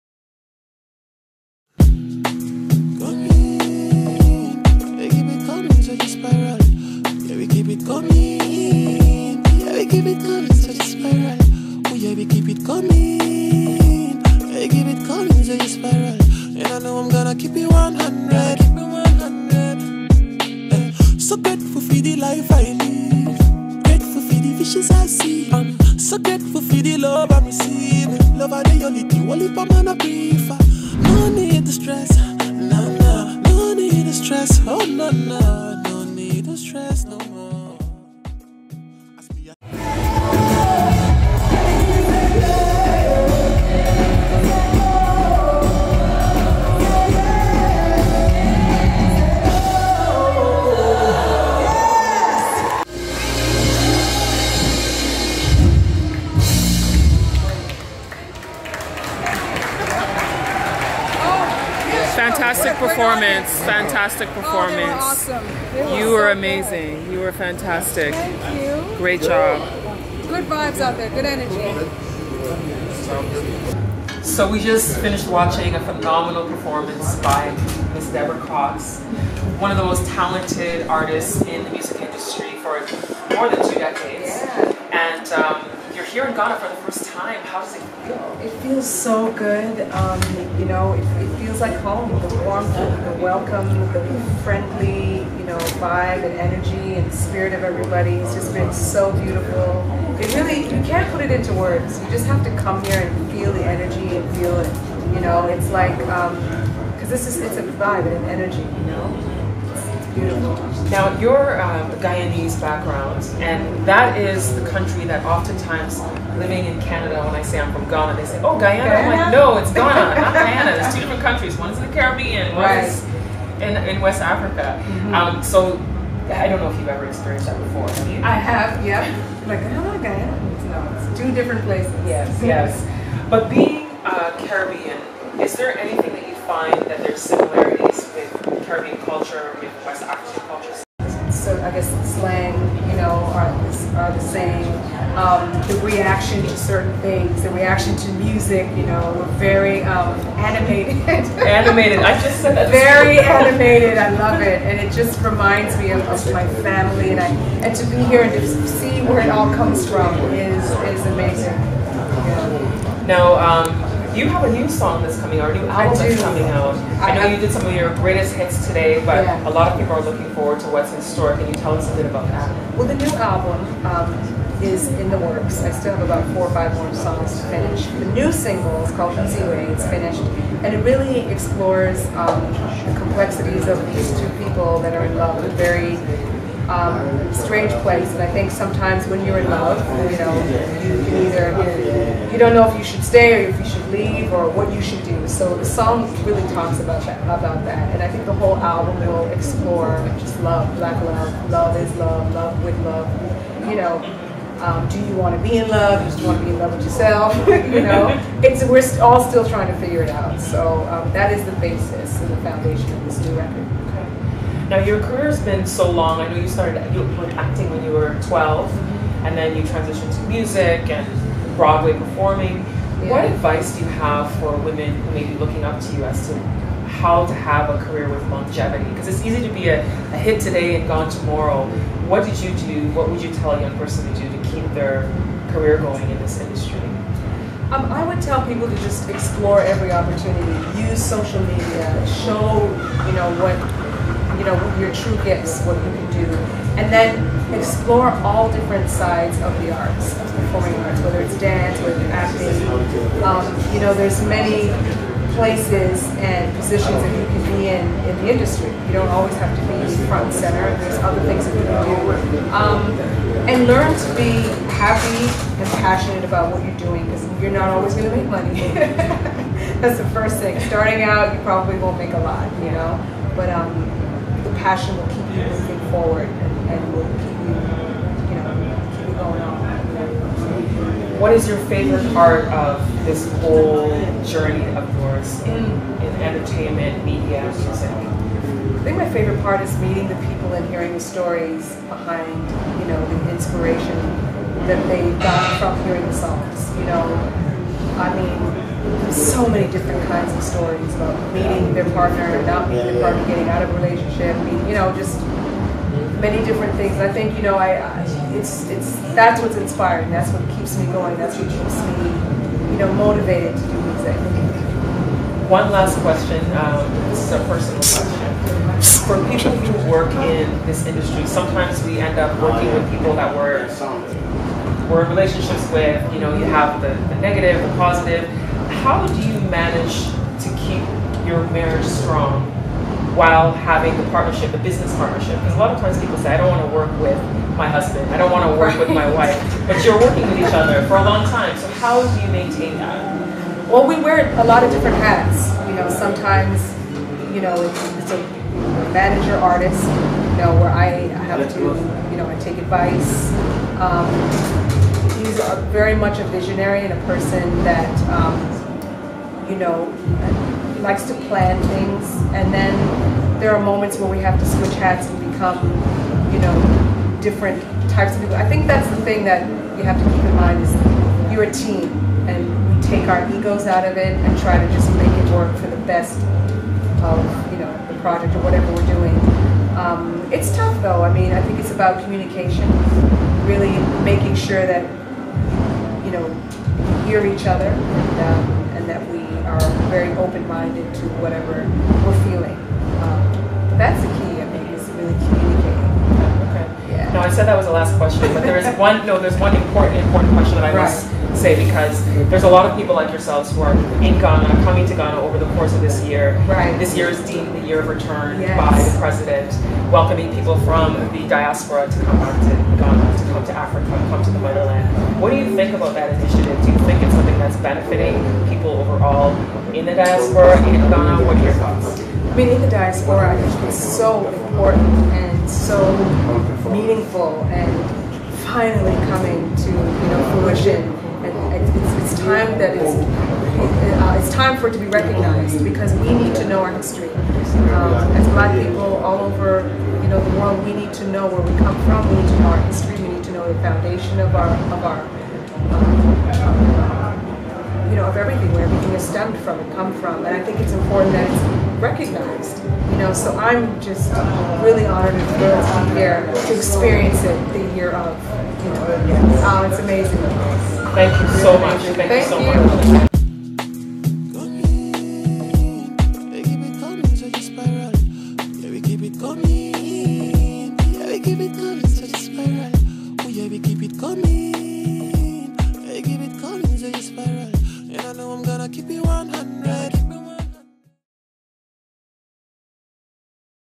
Interview with Deborah Cox at Ghana World Music Festival (Year of Return)
Singer/Songwriter Deborah Cox came to Ghana during the 'Year of Return' and performed at the Ghana World Music Festival.
This is my interview with her after her performance.